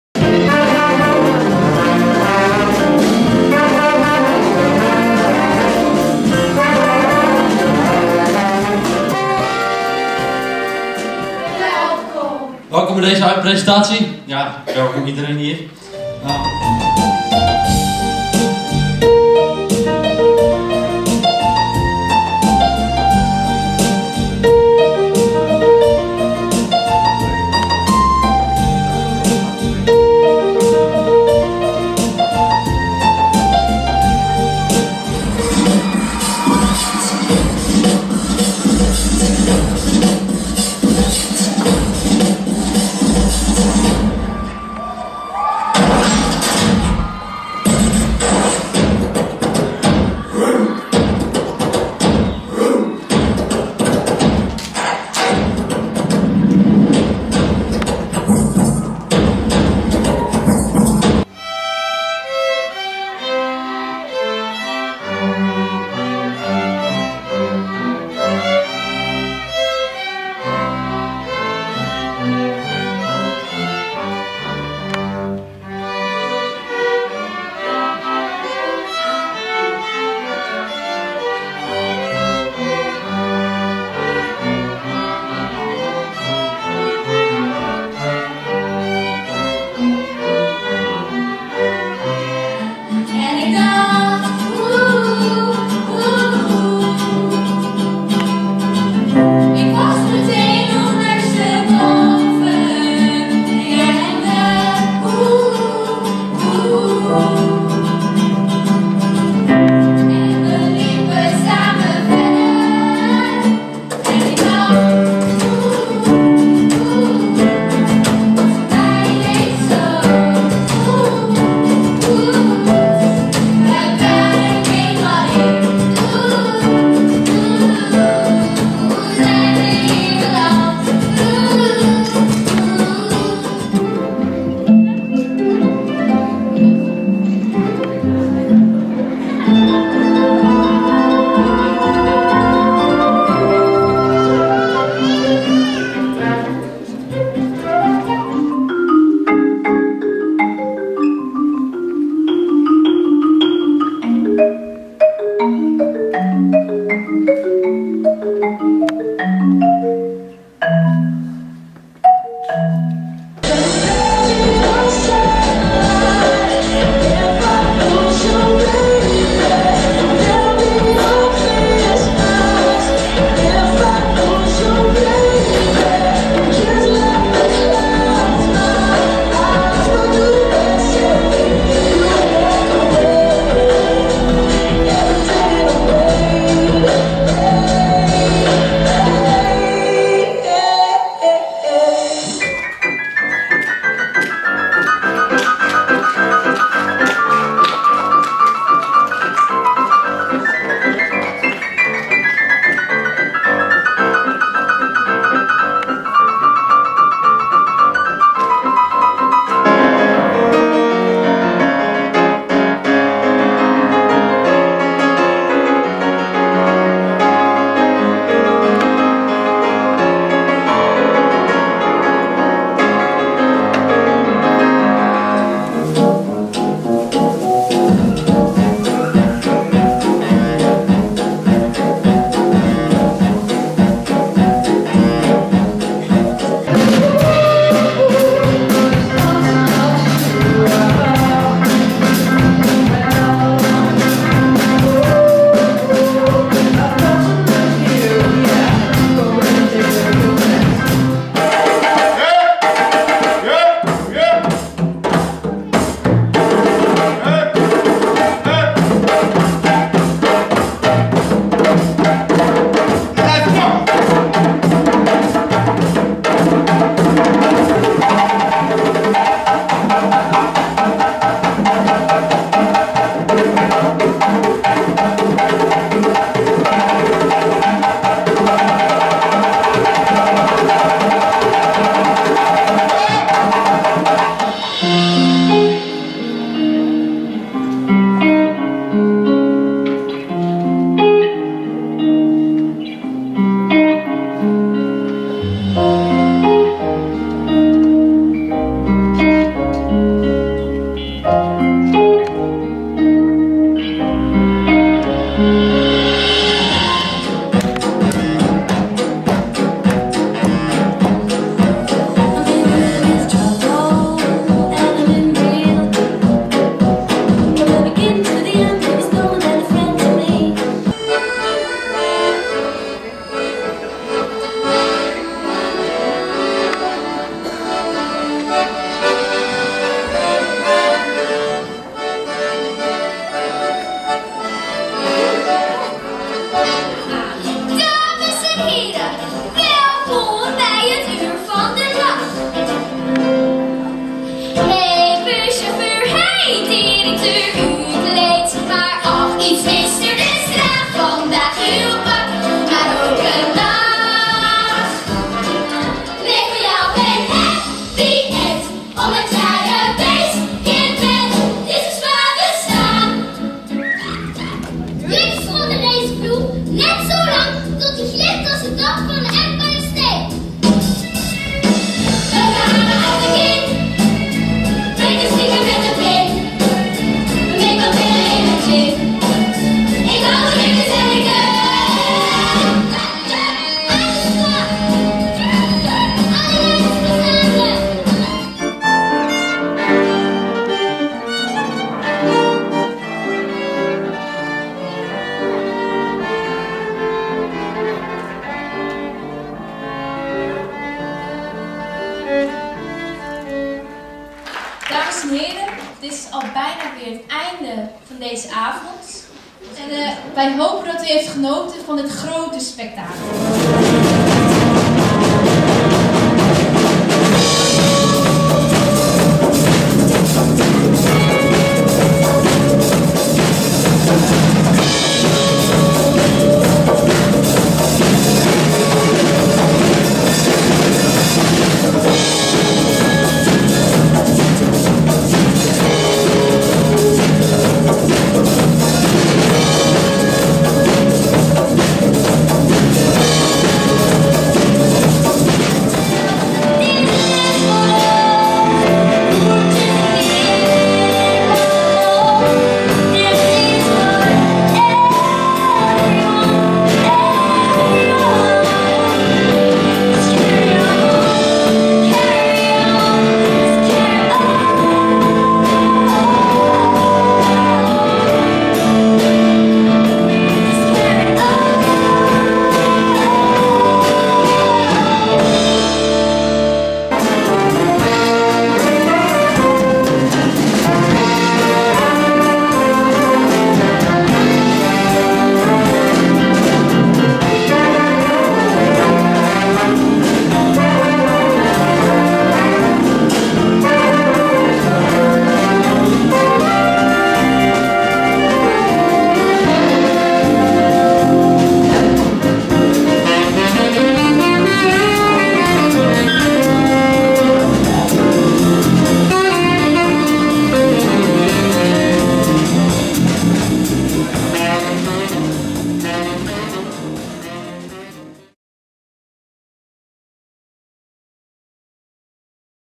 eindpresentatie "i'A.M. ready!"
Eindpresentatie Ateliers Majeur, zaterdag 28 juni 2014, op het podium van het Posthuis Theater Heerenveen: "they were ready!"